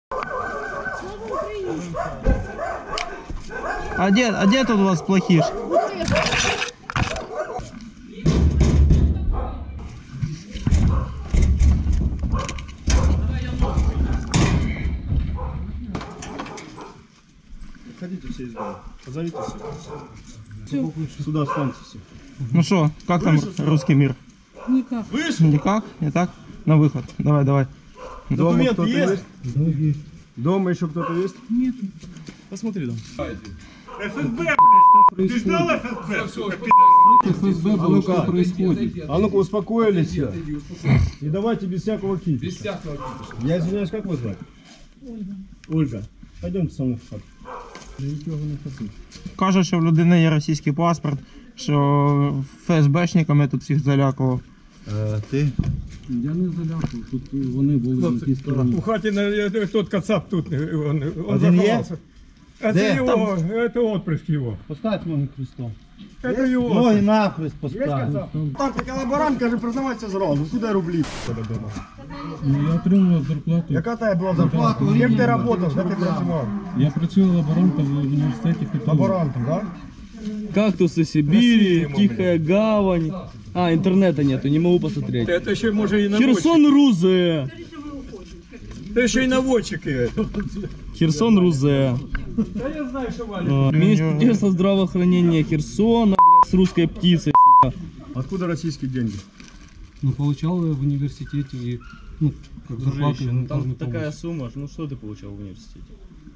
This is the freedom that the Ukrainian armed forces are carrying in Kherson
This is the freedom that the Ukrainian armed forces are carrying in Kherson, all civilians can be accused of being pro-Russian and their fate will not be known